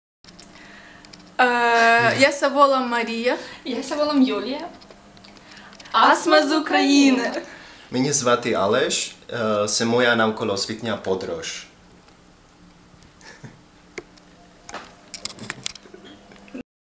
RTW trip: Surfing in Lviv
This is my host Marynka and her flatmate Yulia. I surfed their couch for one day.